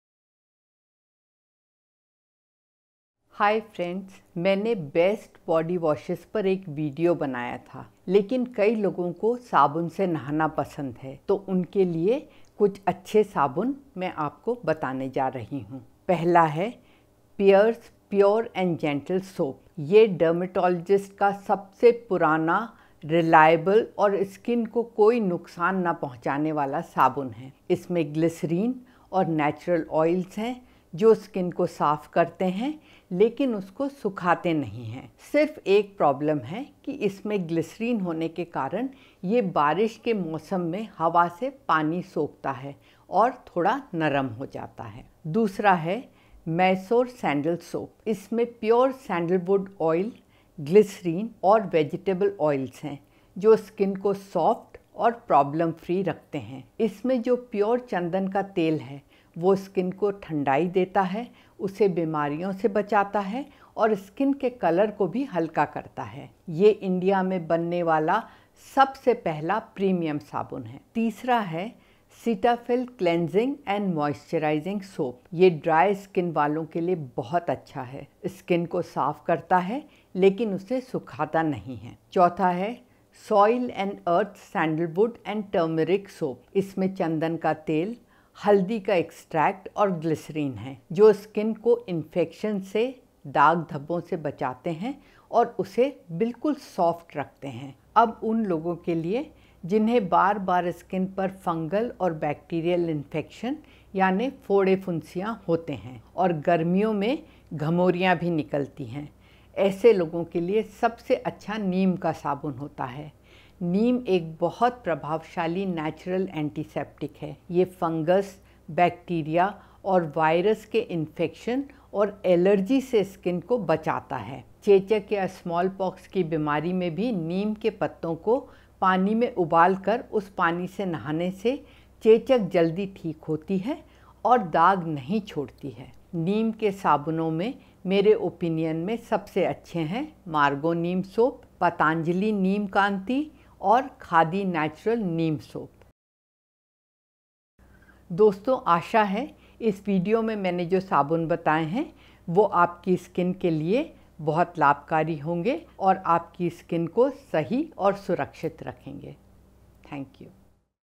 सबसे अच्छे और सुरक्षित साबुन || Best Soaps For All Skin Types
इस video में मैंने सब स्किन टाईप के लिये कुछ अच्छे और सुरक्षित साबुनों के बारे में बताया है।

In this video I have recommended the best bathing soaps for all skin types available in the market.

0:00 Introduction / परिचय
0:17 Best soap recommendations / सबसे अच्छे साबुन
1:42 Soaps for skin with bacterial and fungal infections / डैड और मुहासों से पीड़ित त्वचा के लिए साबुन